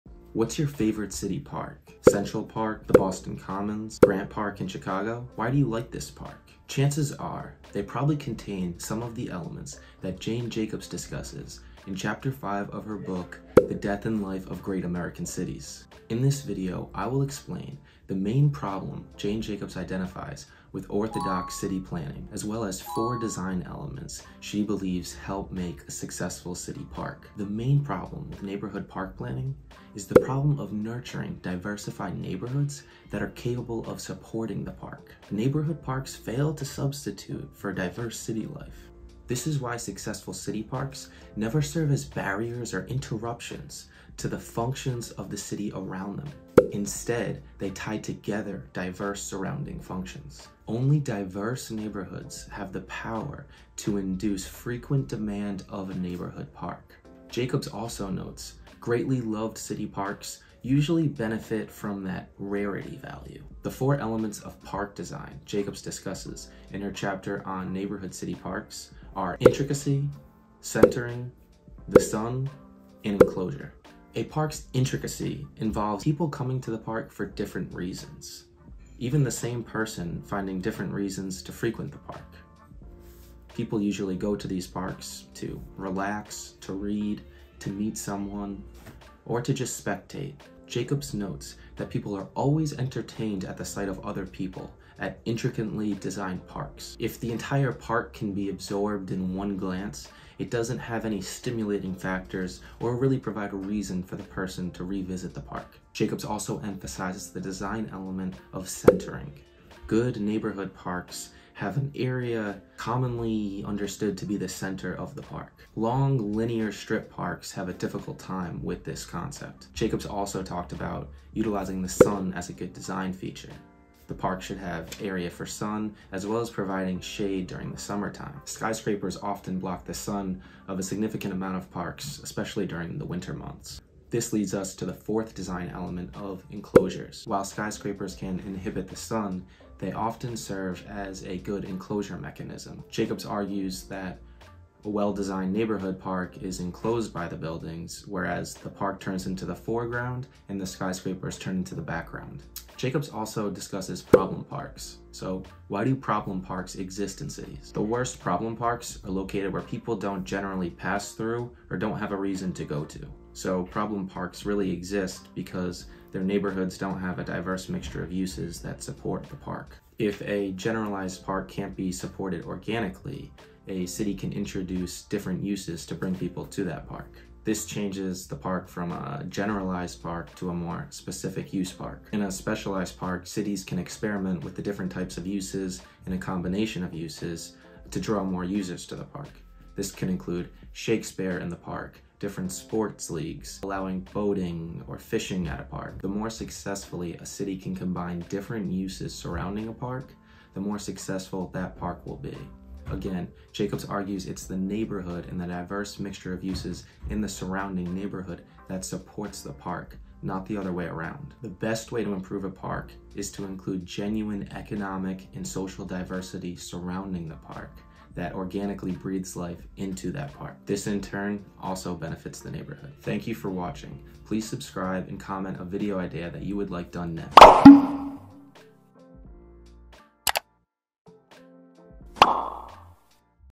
What Makes a Great Park? Jane Jacobs | The Death and Life of Great American Cities
► You can buy her book on Amazon
In Chapter 5 of The Life and Death of Great American Cities by Jane Jacobs, Jacobs discusses the use of neighborhood parks in a city. Jacobs goes into depth describing four elements of a traditional neighborhood park.
1. Intricacy
2. Centering
3. Sun
4. Enclosure

Comment an urban planning topic you would like explored in future videos!